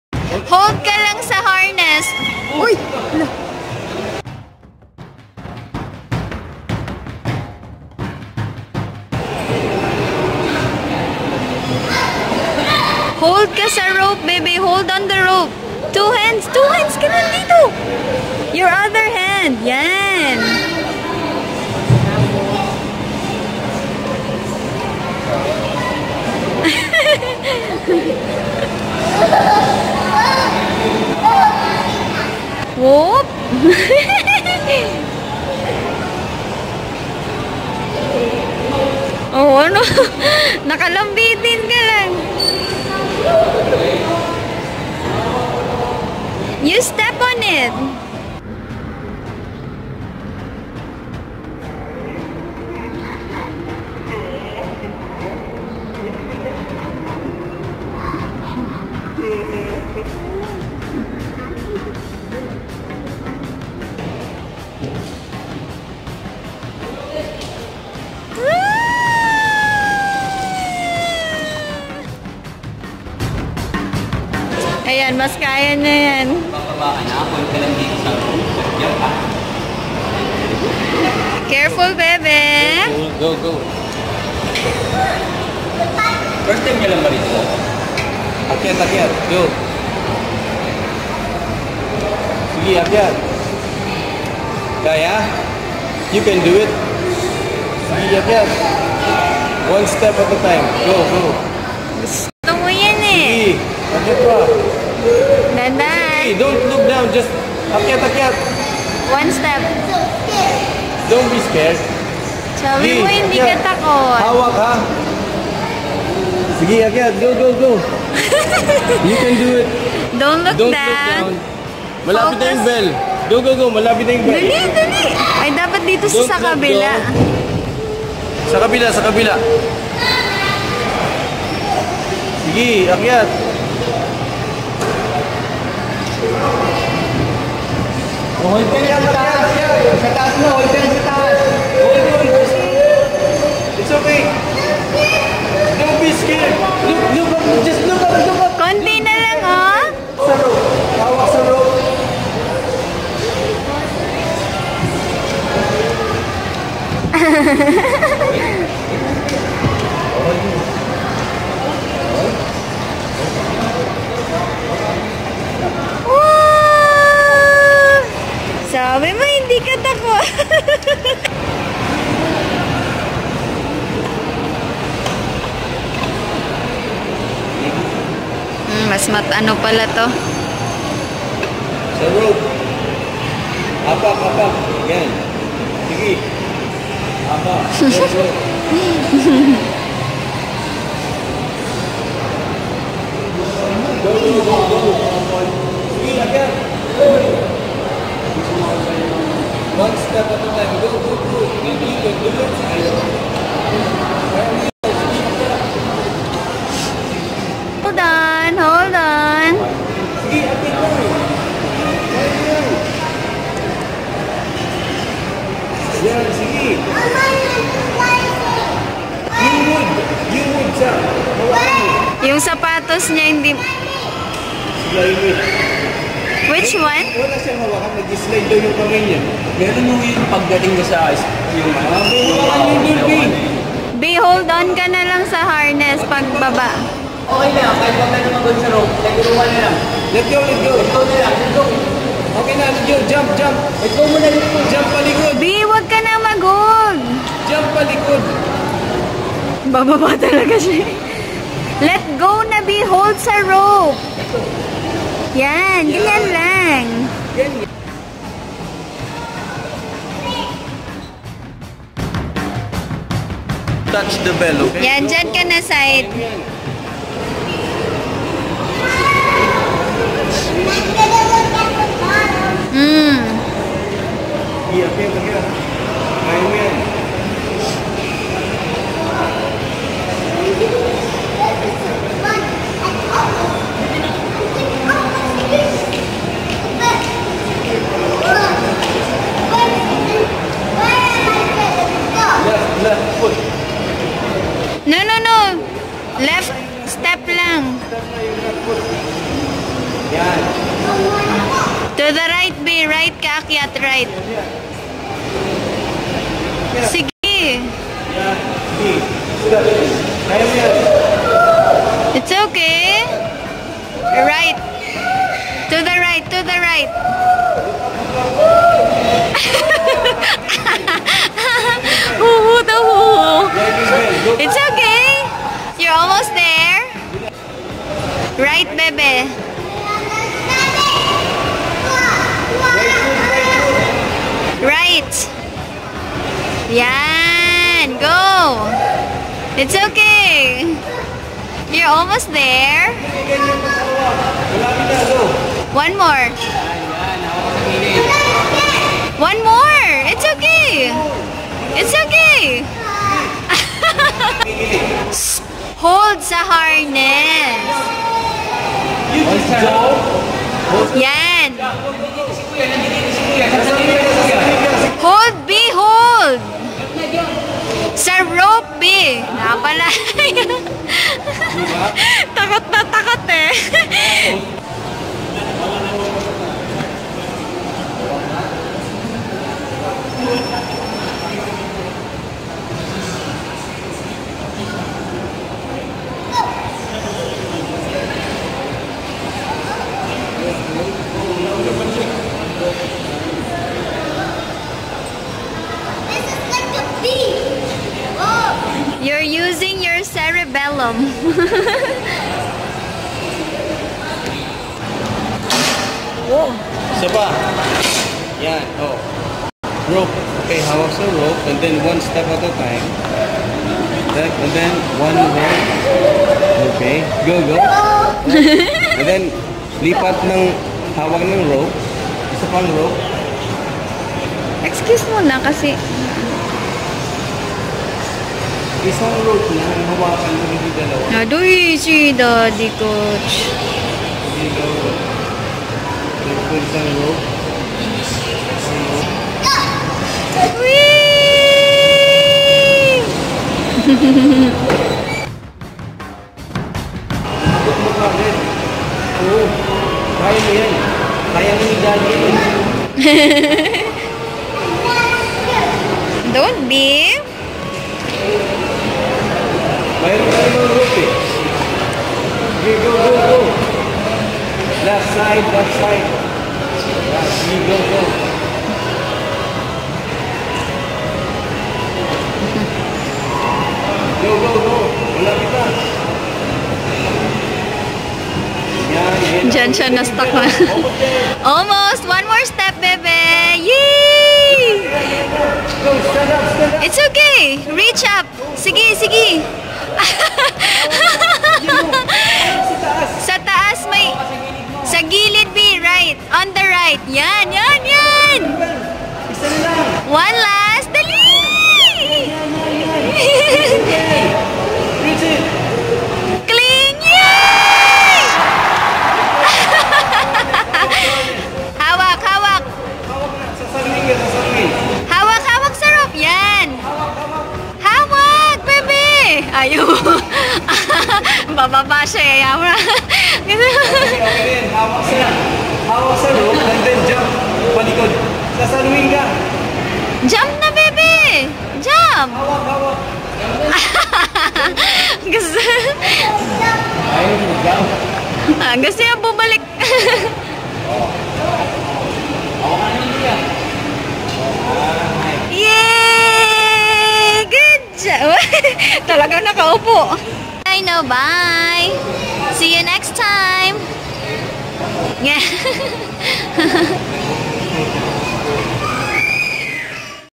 Dani goes on a Jurassic Adventure with a dash of Hugot.😂
Coz she likes challenges... will she ever reach the bell? Watch til the end. 😊❤